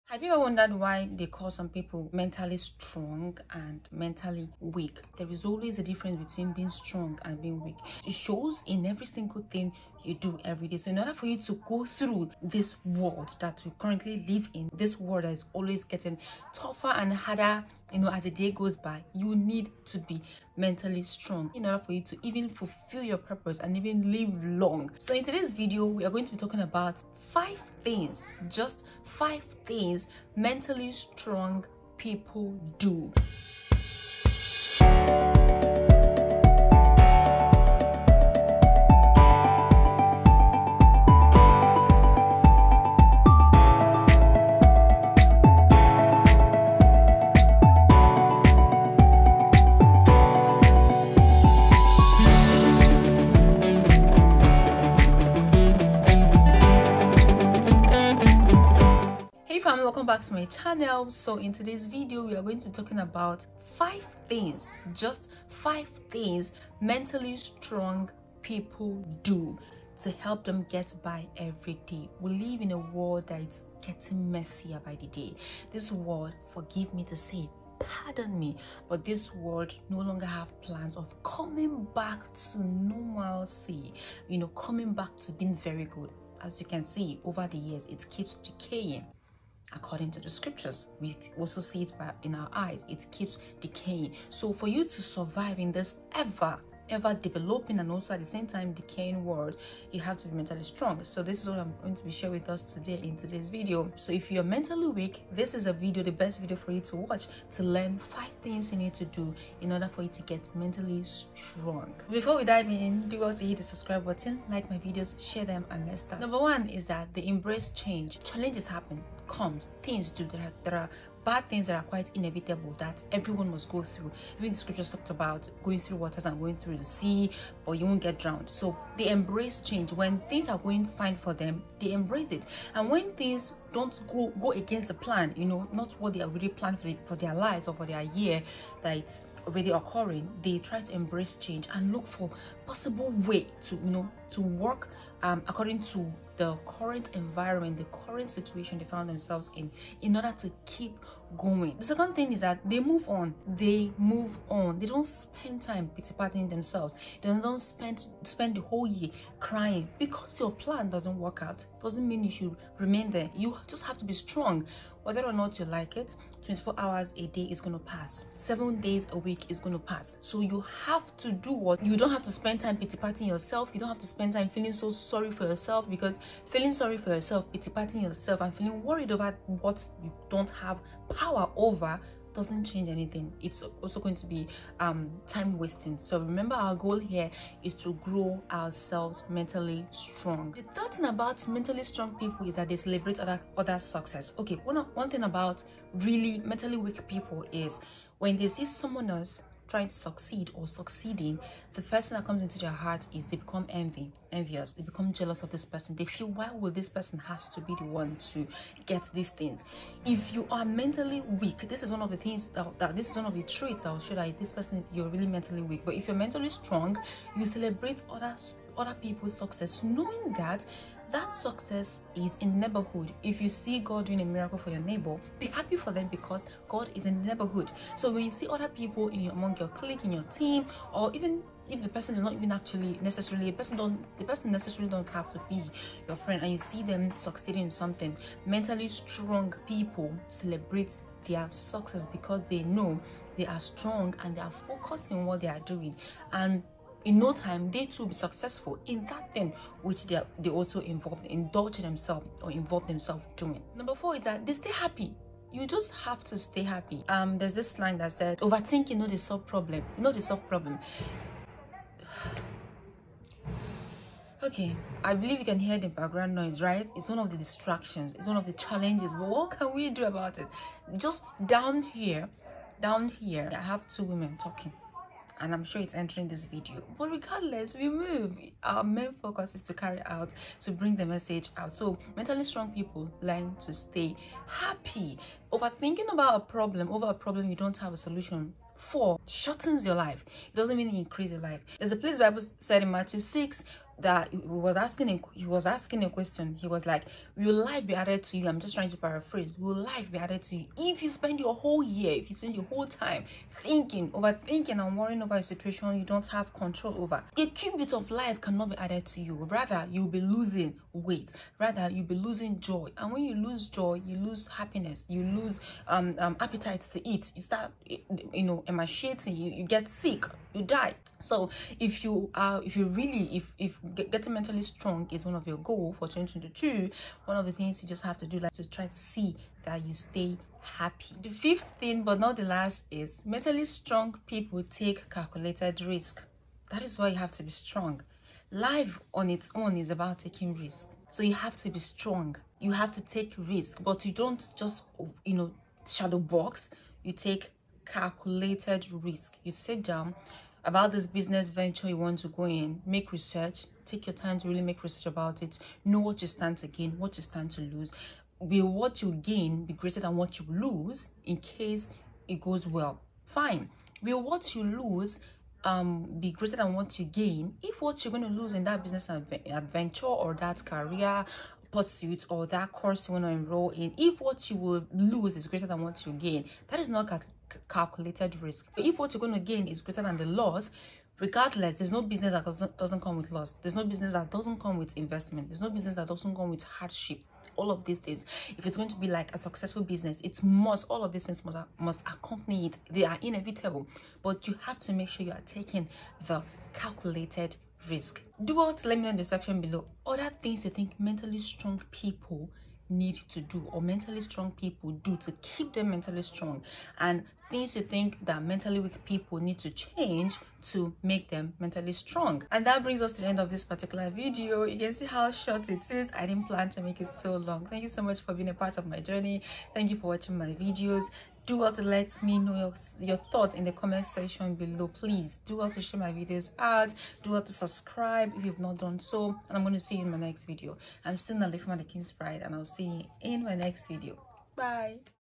5 THINGS MENTALLY STRONG PEOPLE DO IN 2022 TO GET BY CHALLENGES
We live in an ever-developing and at the same time decaying world and in other to survive, you need to be Mentally Strong. I hope this helps.
GOD'S WORD FOR YOU: Deuteronomy 31:6
 "Be strong and courageous. Do not be afraid or terrified because of them, for the LORD your God goes with you; he will never leave you nor forsake you."